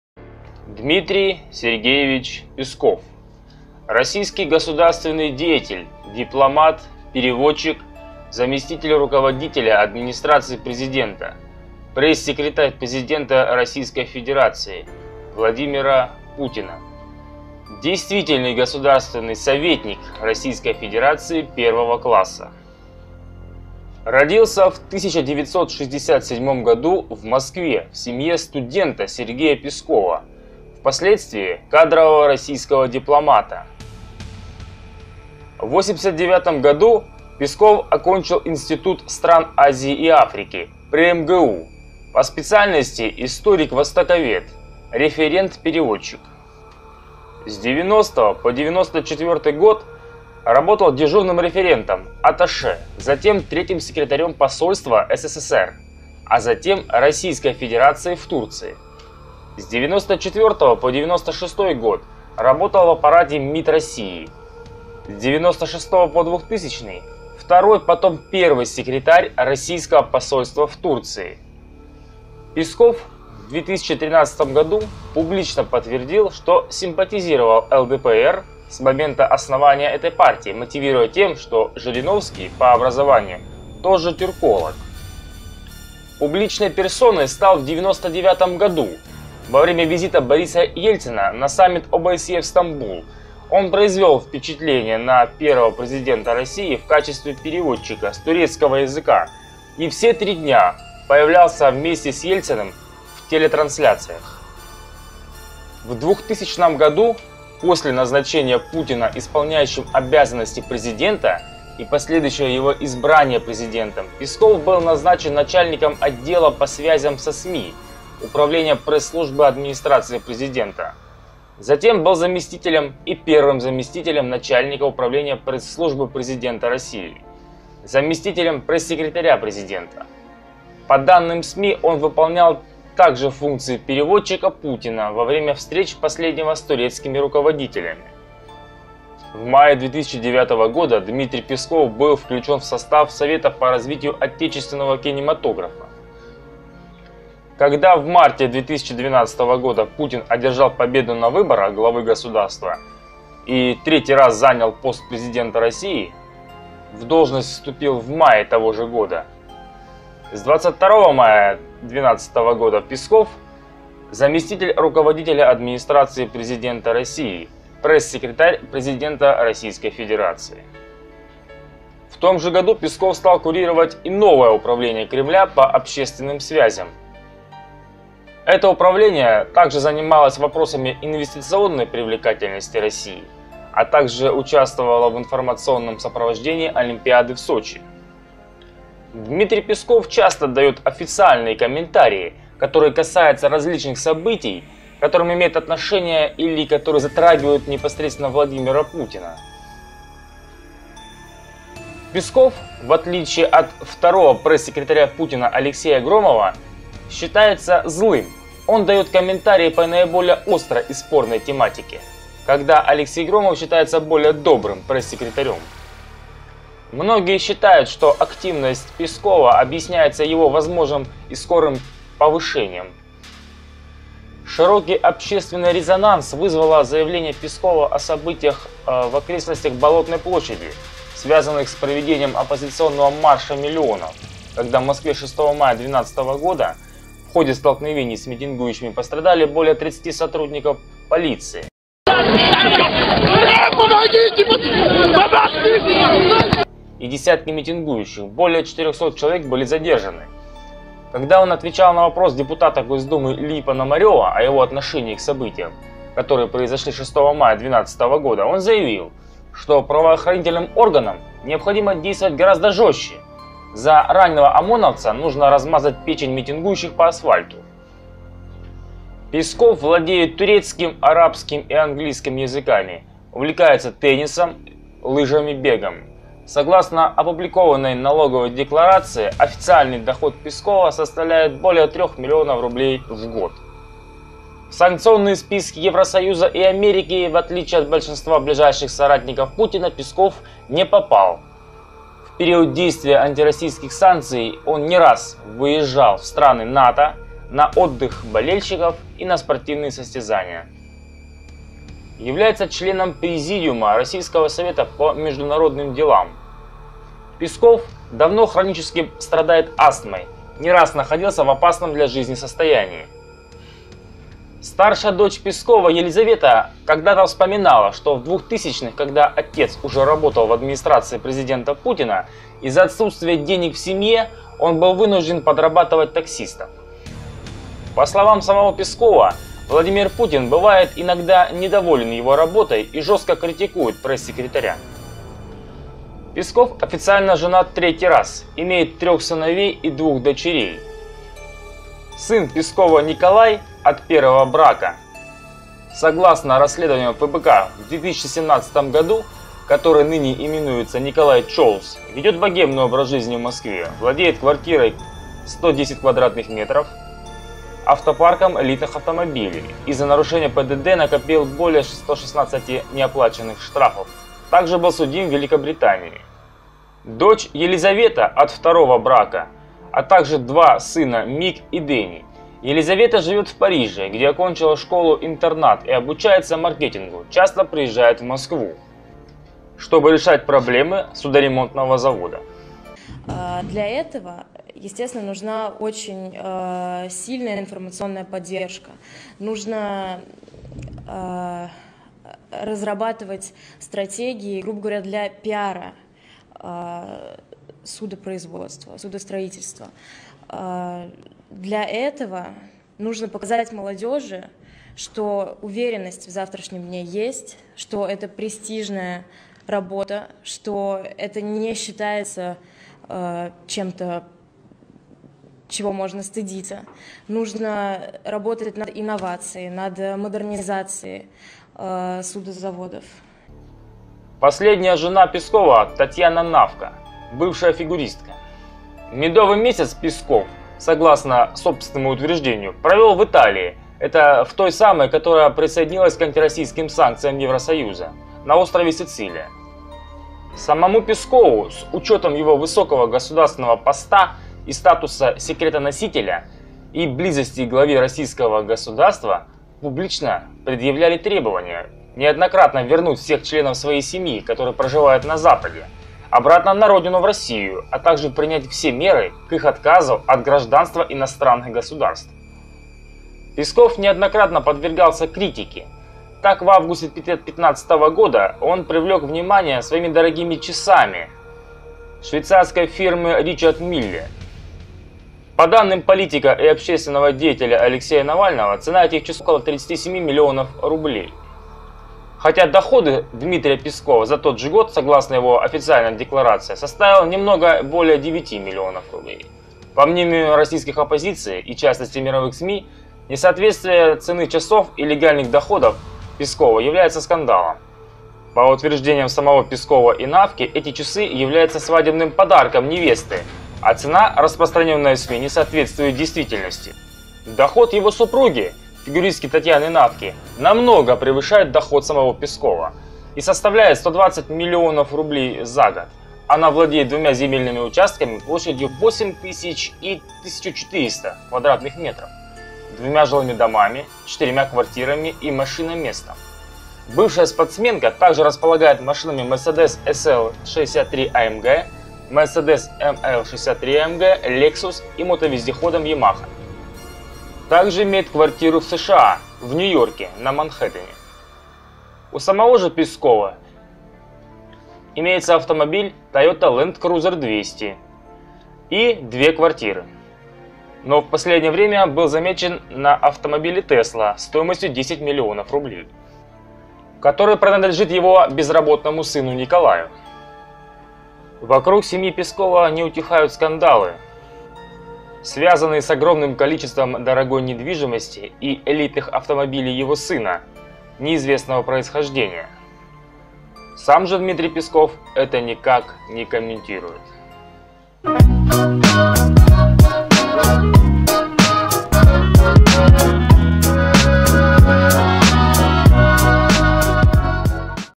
ДМИТРИЙ ПЕСКОВ - КТО ОН?
Кто же такой Песков Дмитрий Сергеевич? Полная настоящая биография пресс-секретаря президента Российской Федерации Дмитрия Пескова в этом видео.